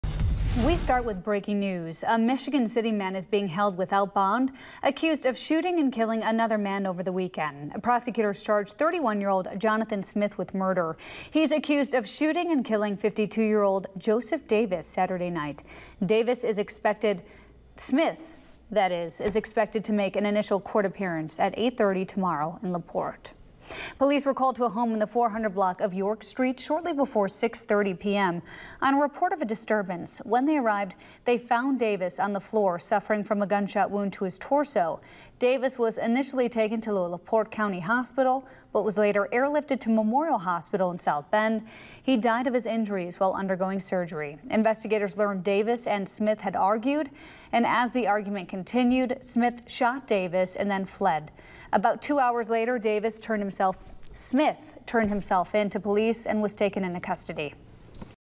Michigan City man charged after shooting, killing another man during argument
Jonathan Smith is currently being held at the LaPorte County Jail without bond pending his initial hearing.